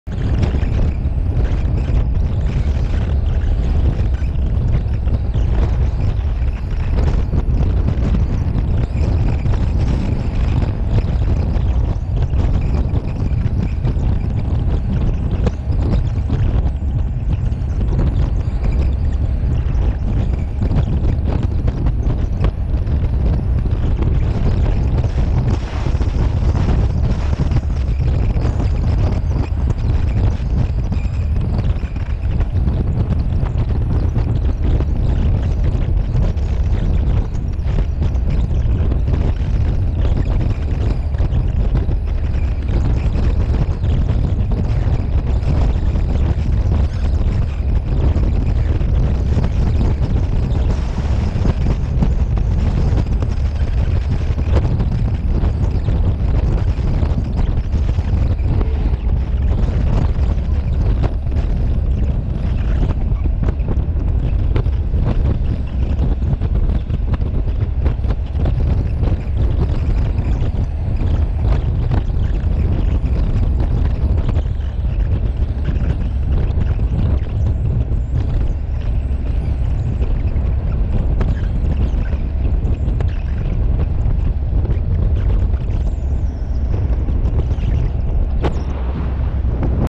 TESTANDO A GOPRO 360 NA ESTRADA COM A TIGER, LOGO MAIS VEM O VIDEO DA SERRA DO RIO DO RASTRO EM 360º
GIRE O VIDEO COM O MOUSE OU SE ESTIVER VENDO COM O CELULAR MOVIMENTE O CELULAR PARA ASSISTIR EM 360º, MELHORE A QUALIDADE NOS AJUSTES E COLOQUE PELO MENOS EM 1080S HD